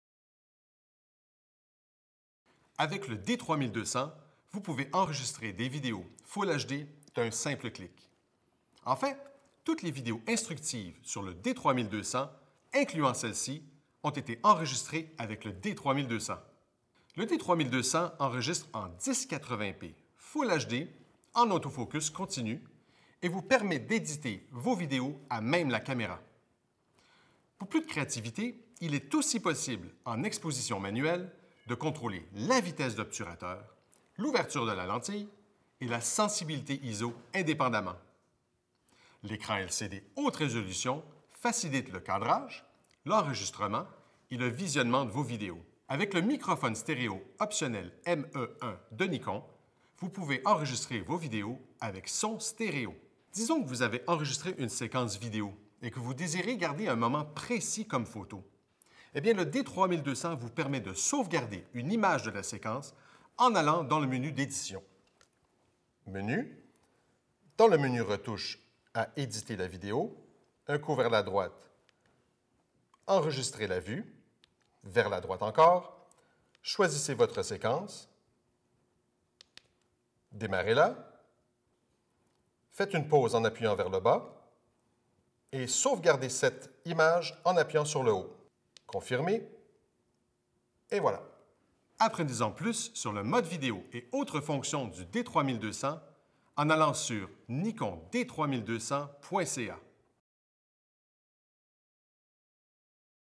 Série de vidéos « Comment faire » : Vidéo HD avec le reflex numérique HD Nikon D3200
Sur cette vidéo, Stéphane Bédard, technicien spécialisé chez Nikon Canada, présente la fonction vidéo full HD 1080p du D3200 avec mode D-Movie permettant de passer de l'enregistrement vidéo full HD au déclenchement de photos. Stéphane explique également comment sauvegarder une image extraite d'un film en fichier jpeg.